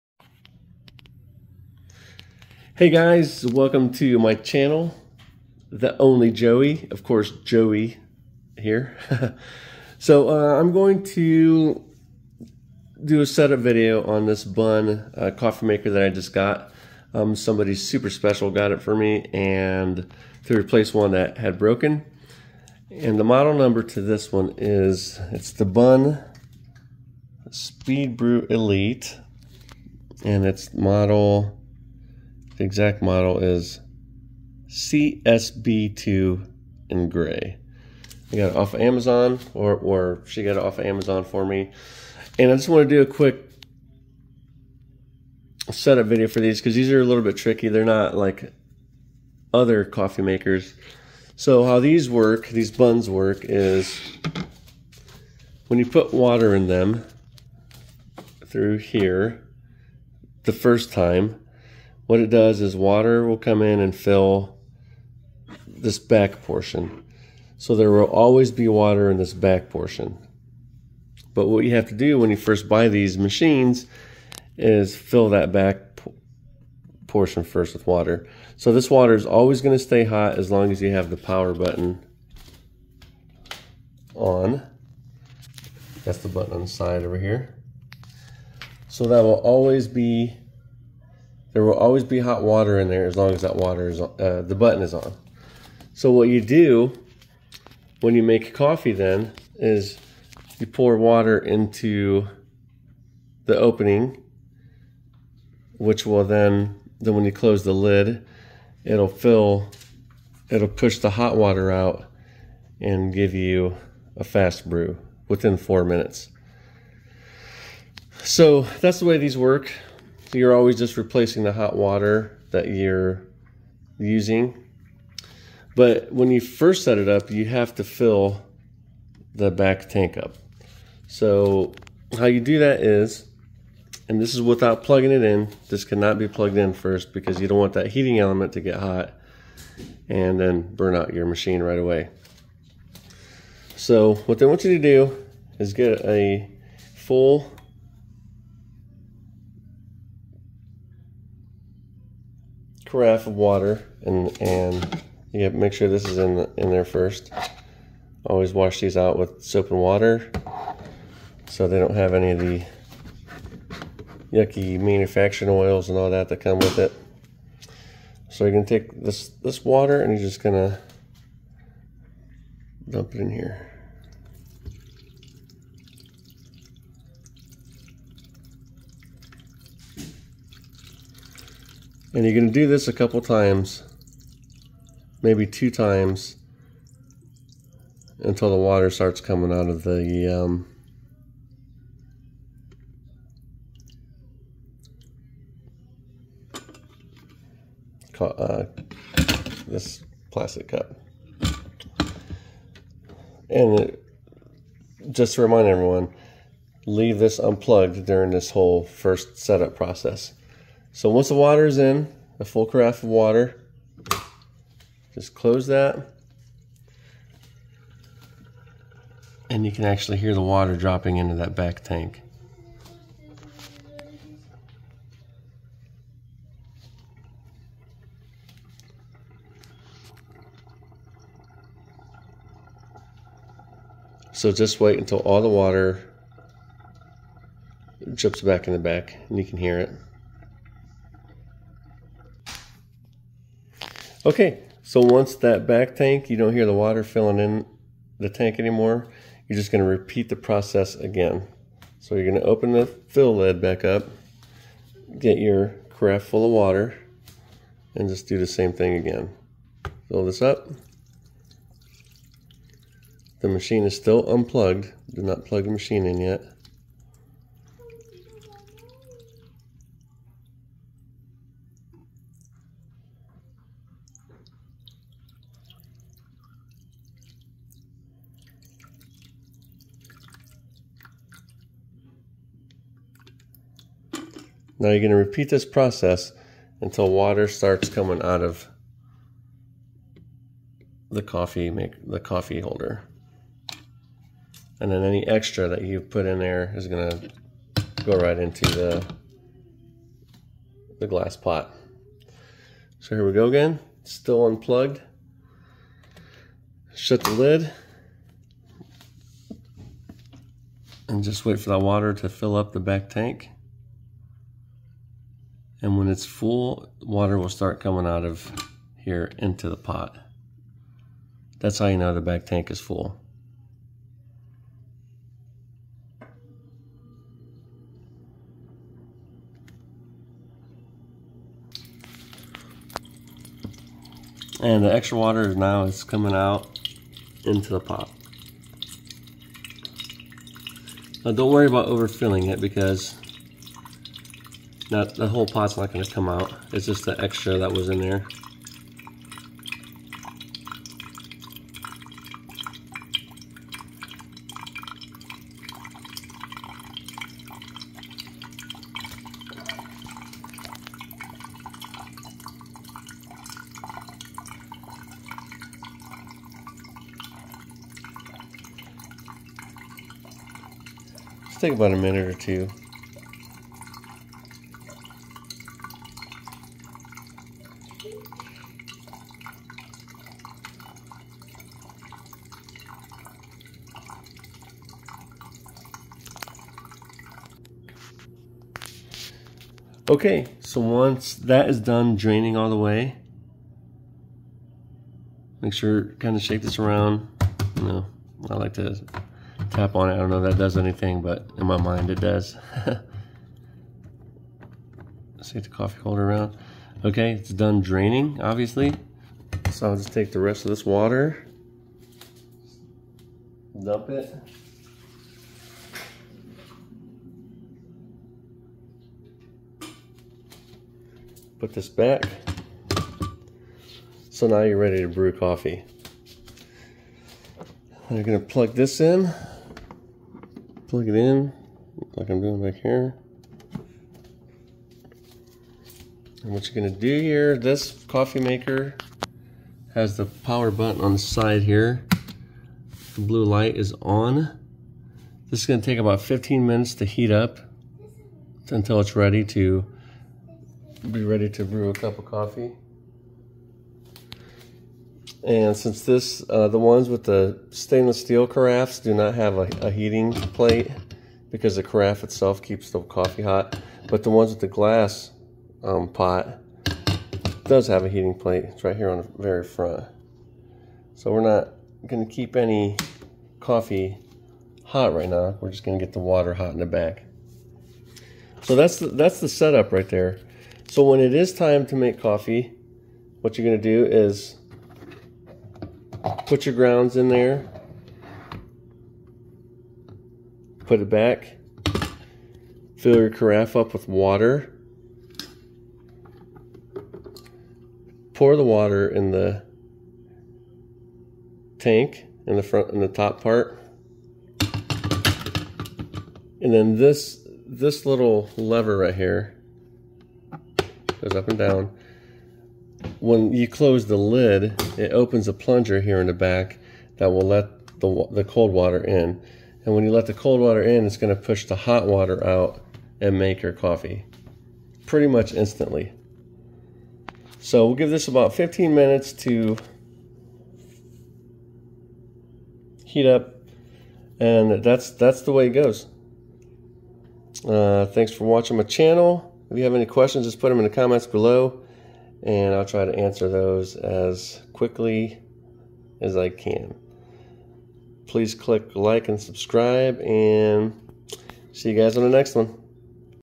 BUNN Speed Brew Elite (CSB2) initial set-up
This is an initial set up for most Bunn coffee machines. I use about a half cup of tap water and the rest distilled water to help prevent calcium and mineral build up in the machine.